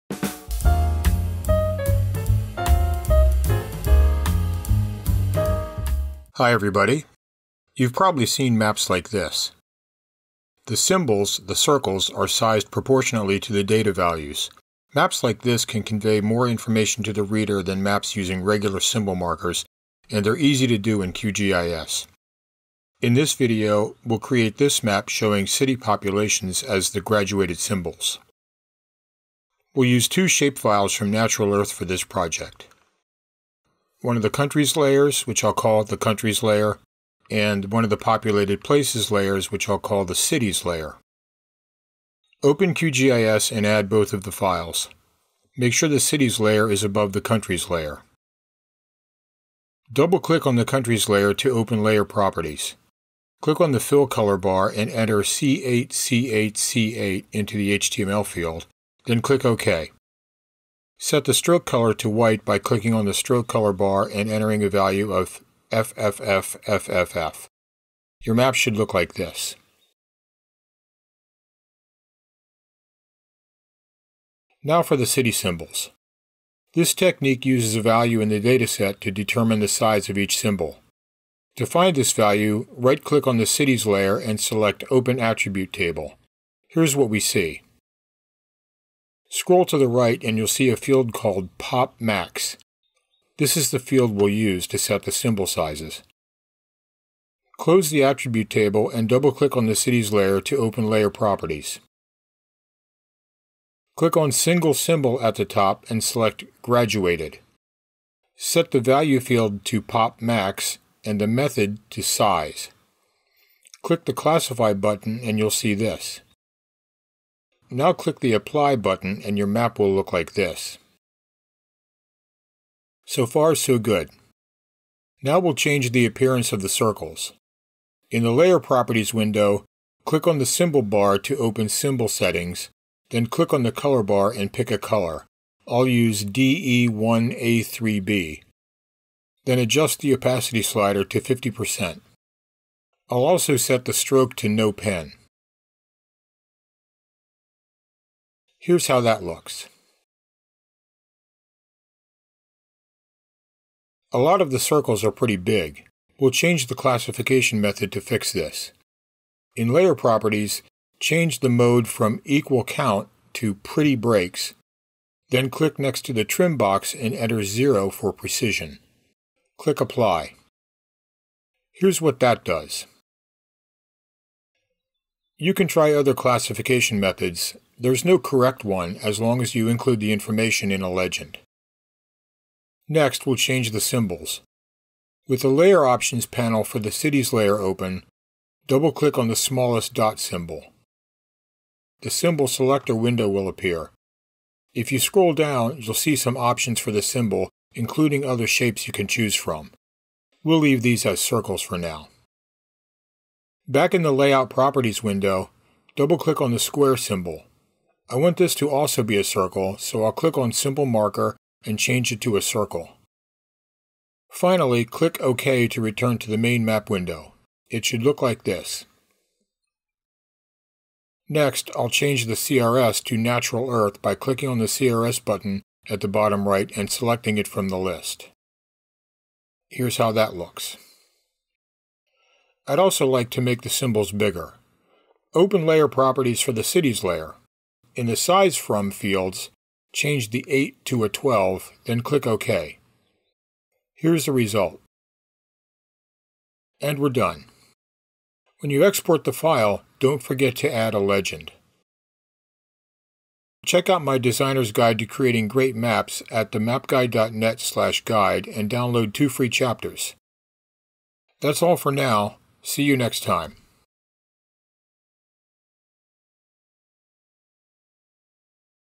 Using Graduated Symbols in QGIS
This video covers how to use graduated (categorized) symbols in QGIS instead of standard markers. This is an easy way to add visual interest and additional explanatory power to a map.